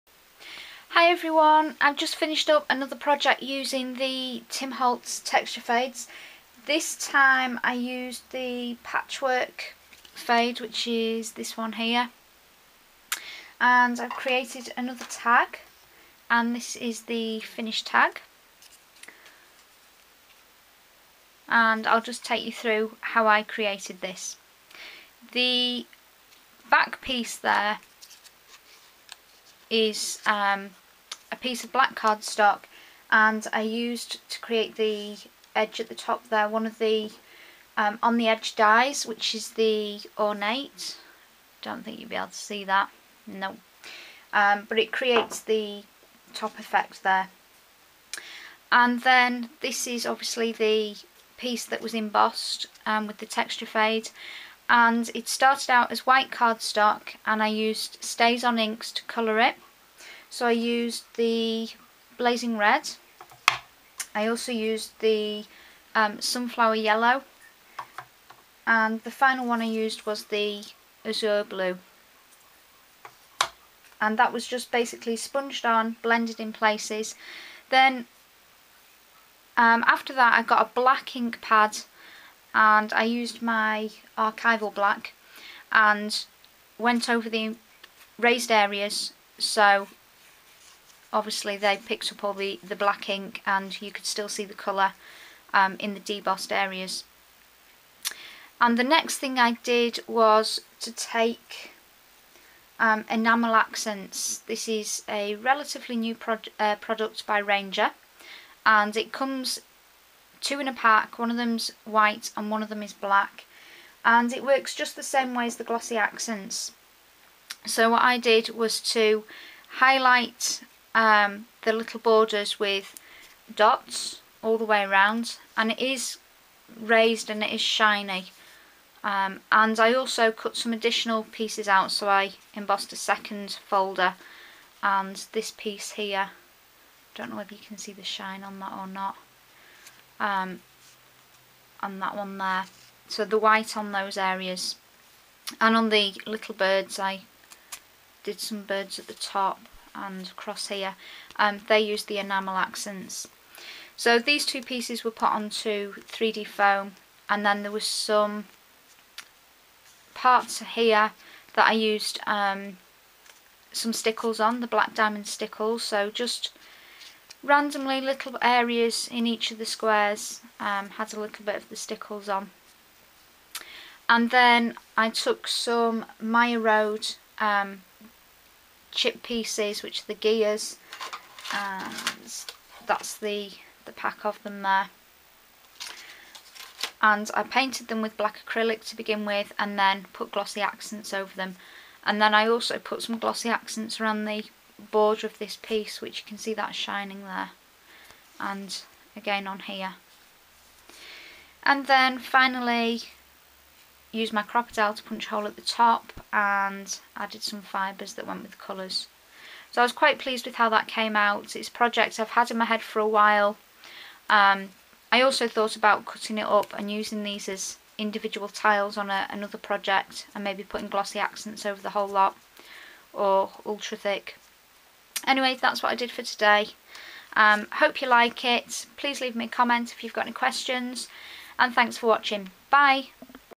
Tim Holtz Patchwork Tag
A tag project using the Tim Holtz Patchwork Texture Fade, alongside StazOn inks & Enamel Accents